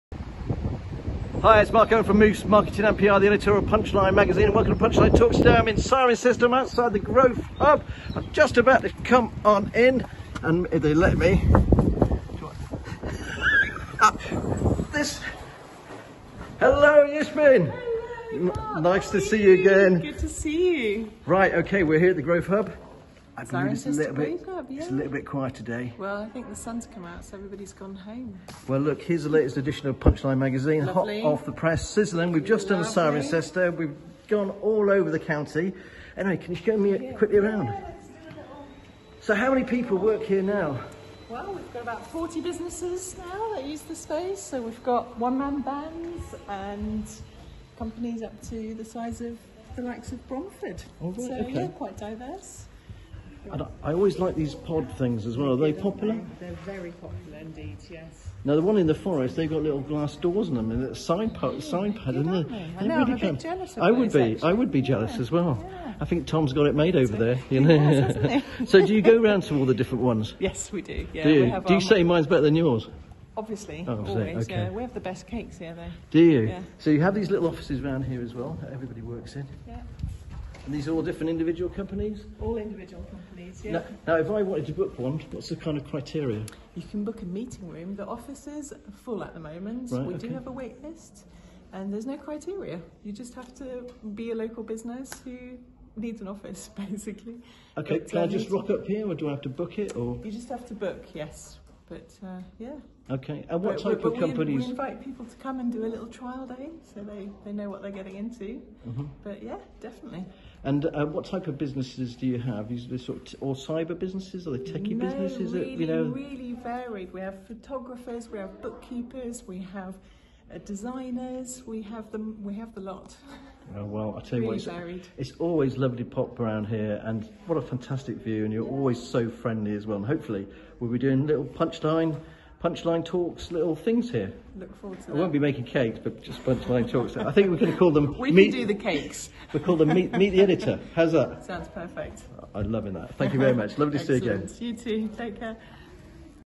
Punchline delivered their latest magazine at Growth Hub Cirencester & met up with Yesim Nicholson.
The Punchline-Gloucester.com team delivered their latest magazine at Cirencester's Growth Hub and met up with their manager Yesim Nicholson. Here's a quick tour of the Hub, once they let us back in!

Punchline magazines are now available at all the Gloucestershire Hub's (April 21, 2022) so please pop along and pick up a free copy. Gloucester, Stroud, Forest of Dean, Cirencester and Tewkesbury.
Magazines can be found in the Cotswolds Library GFirst LEP Growth Hub sections - but please go along quickly as they tend to get snapped up.